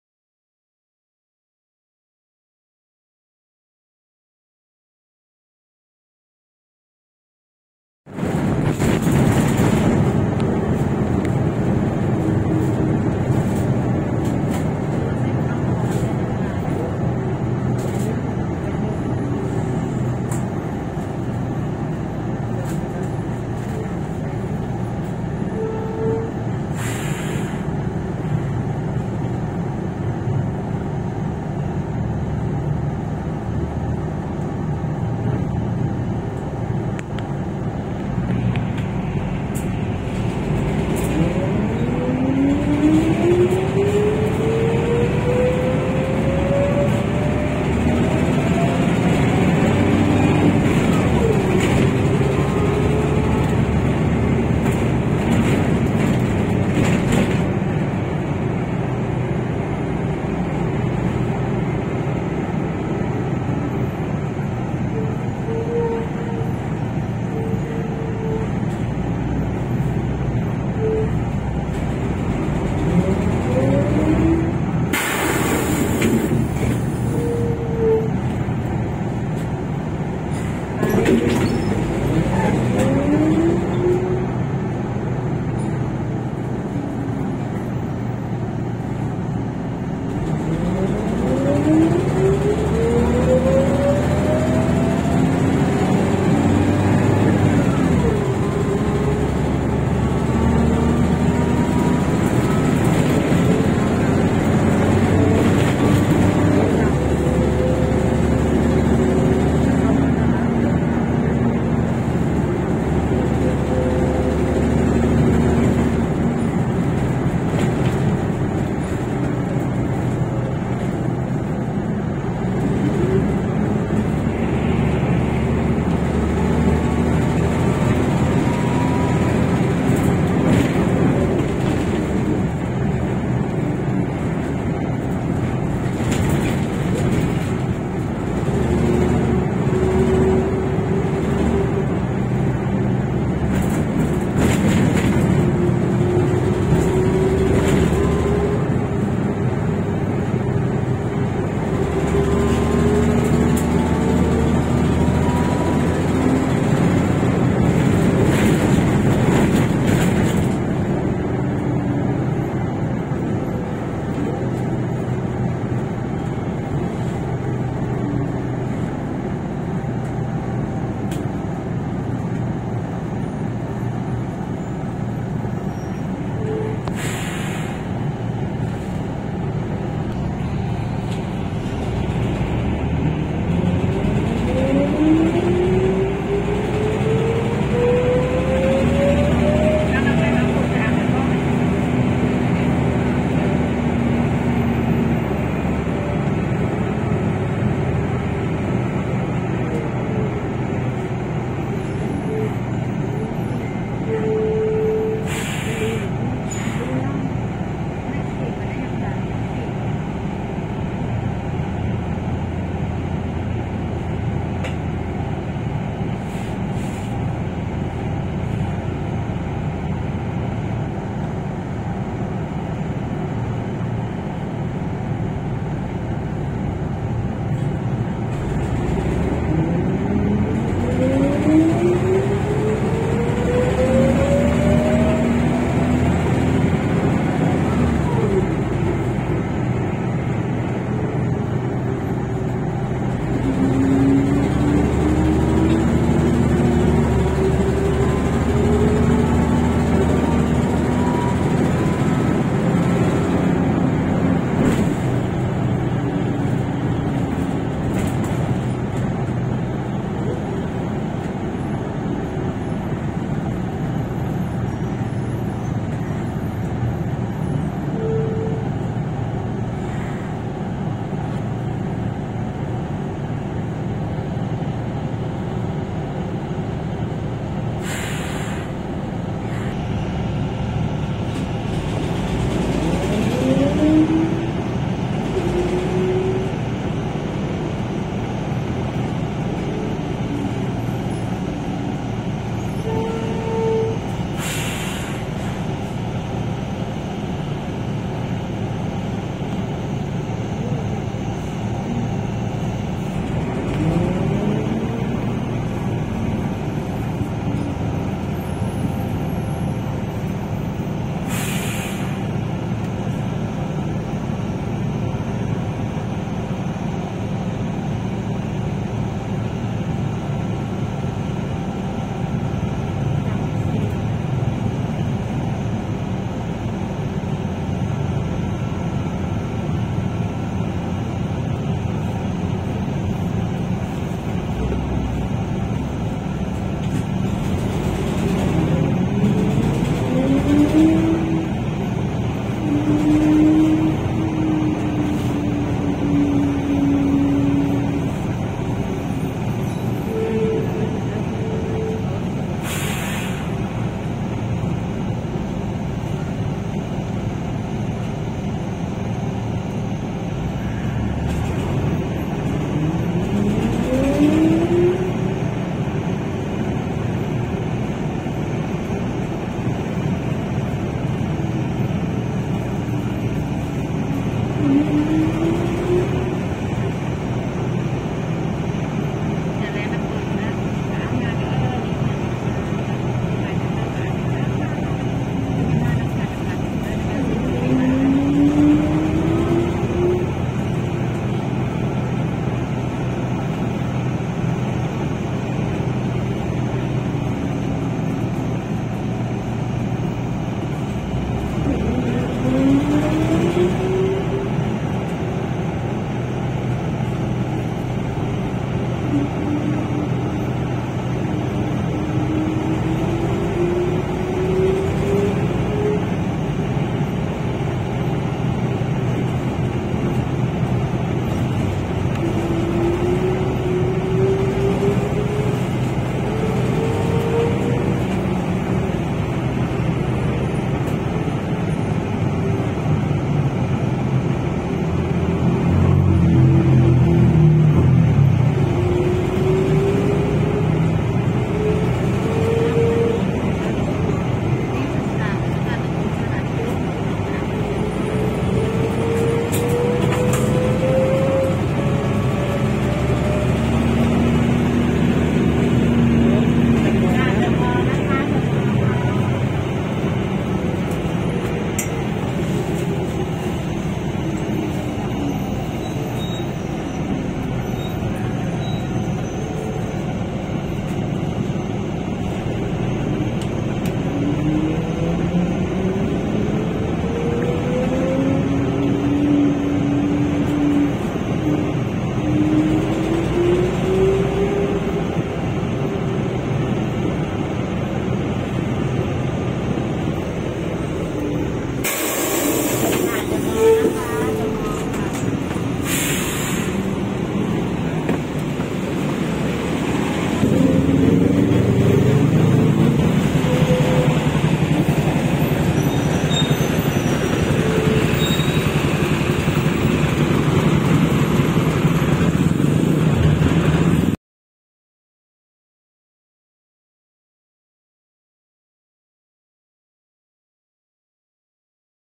[BMTA Bus Zone 2] Hino HU3K Bus Line 502 2-4050 Ep.2 (1080p & 60fps!!!)
รถเมล์ขสมก. เขตการเดินรถที่ 2

ปอ ฮีโน่ครีมน้ำเงิน รุ่น HU3K
สาย 502 เลขข้างรถ 2-4050 ตอนที่ 2
ช่วงลาดพร้าว 101 - แยกแฮปปี้แลนด์/เอ็นมาร์คพลาซ่า

คบิปนี้ถ่ายเมื่อ 5 มี.ค. 65
By Admin Earth_55143